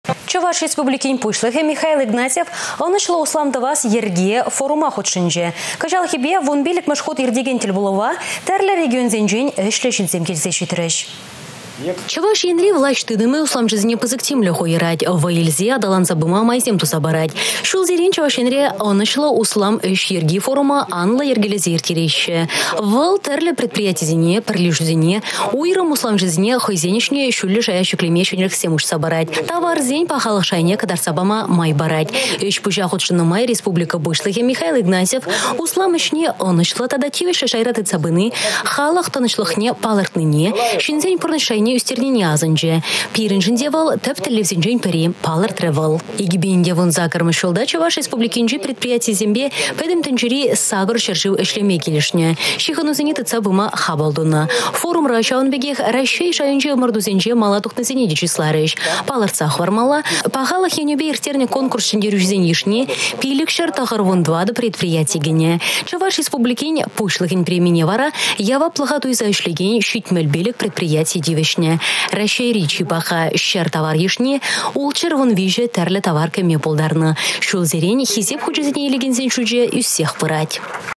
«Ăнăҫлă услам йĕрки» форум
Чăваш Республикин Пуҫлăхĕ Михаил Игнатьев «Ăнăҫлă услам тăвас  йĕрке» форума хутшăнчĕ. Кăçалхипе 15-мĕш хут иртекен тĕлпулăва та тĕрлĕ регионсенчи ĕçлĕ çынсем  килсе çитрĕç